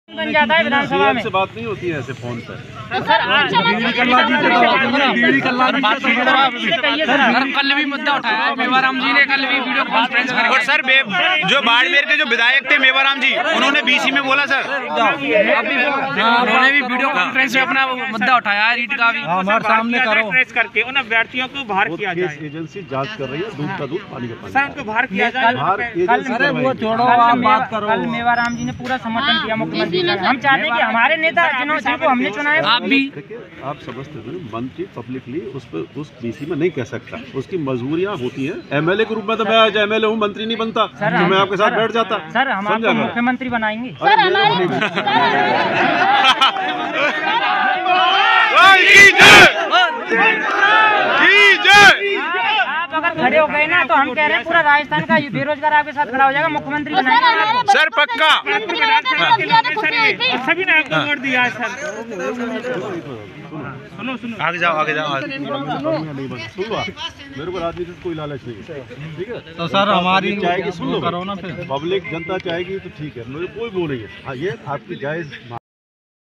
reet2021 Bharatpur के मंत्री सुभाष गर्ग को छात्रों ने कहा "आपको CM बना देंगे,"reet पद 50हजार करवाओ!
भरतपुर में मंत्री सुभाष गर्ग जी से मिले रीट अभ्यर्थी मंत्री ने सभी की बातों को सुना
और रीट के पदों में 31000 से बढ़ाकर 50000 का खुला समर्थन किया और सोमवार को शिक्षा मंत्री जी से मुलाकात करवाने के लिए और सभी बेरोजगारों को यह आश्वासन दिया कि जल्द ही रीट के पदों में बढ़ोतरी के लिए वह मांग पुरजोर तरीके से उठाएंगे.